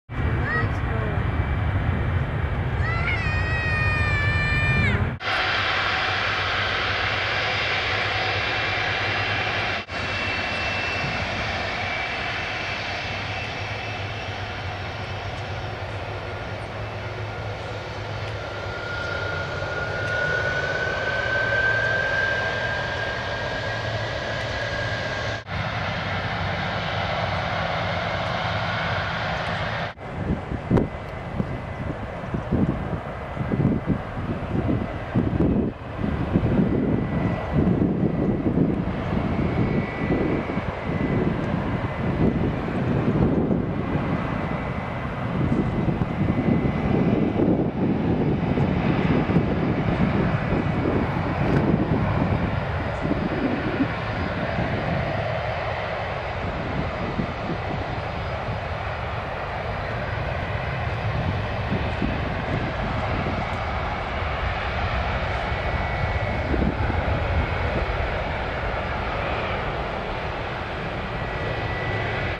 British Airways 747 takeoff from SFO
British Airways Boeing 747-400 takeoff from SFO. Route:

Departure: San Francisco International Airport (KSFO-SFO)

Destination: London Heathrow (EGLL-LHR)

Distance: 5,359. Time: 10 hours 14 minutes.